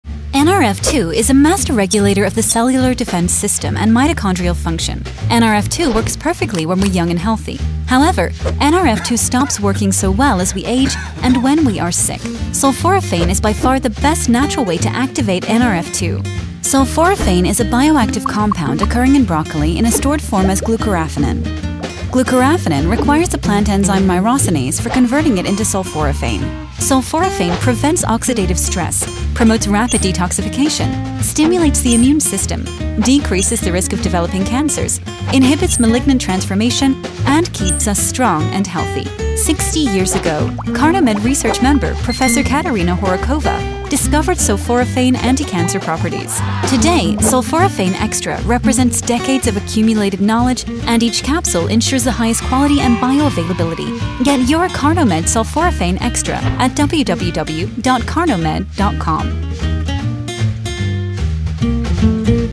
What is Sulforaphane? The cutting edge anti-cancer nutraceutical (2020)
We all know that broccoli is healthy and should be consumed regularly.

But, did you know what makes it a super-food? Sulforaphane!

Sulforaphane is a biologically active compound scientifically extracted from broccoli.

Sulforaphane Extra represents decades of accumulated knowledge and each capsule ensures the highest quality and bioavailability.